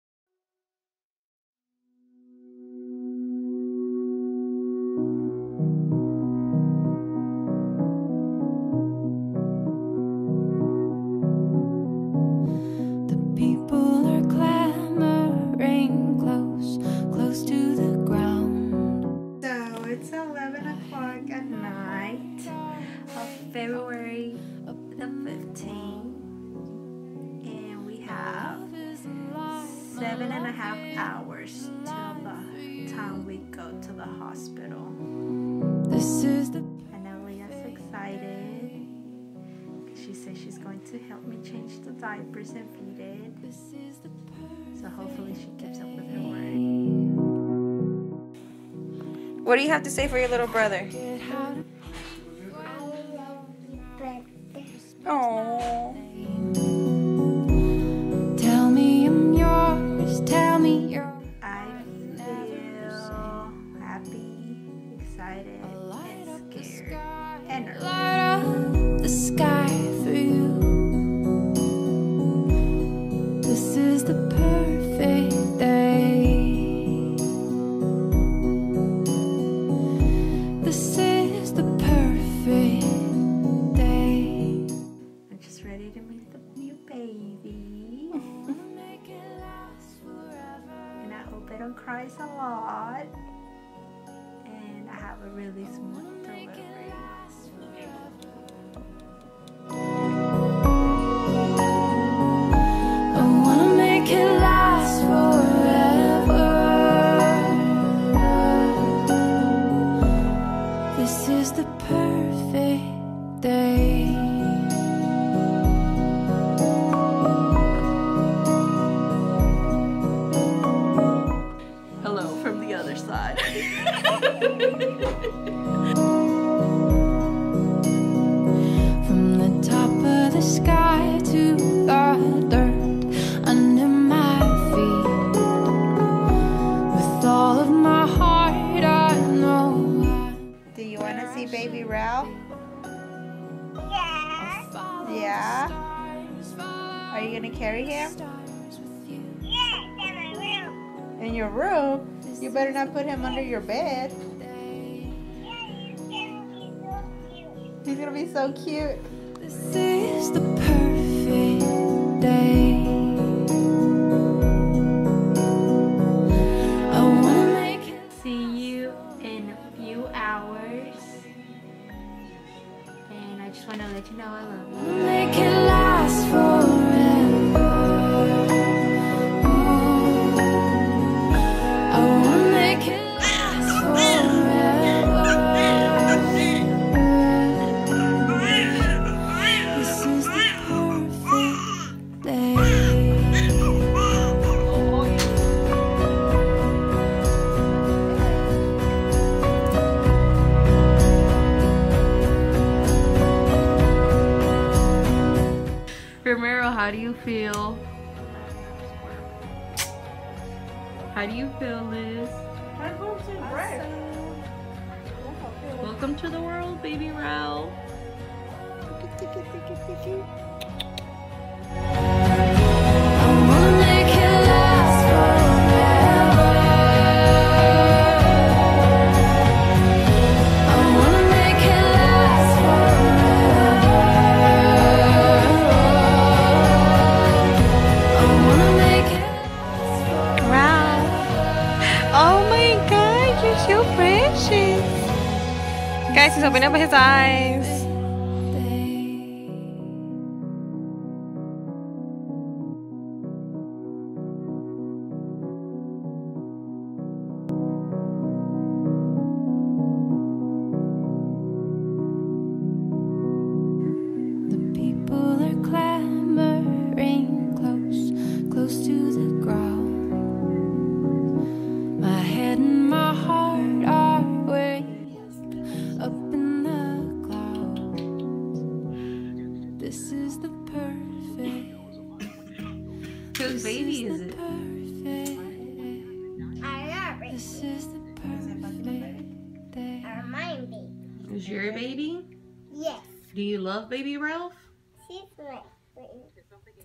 Welcome Baby Ralph
Montage of Liz C's pregnancy and labor, getting ready to meet her new baby boy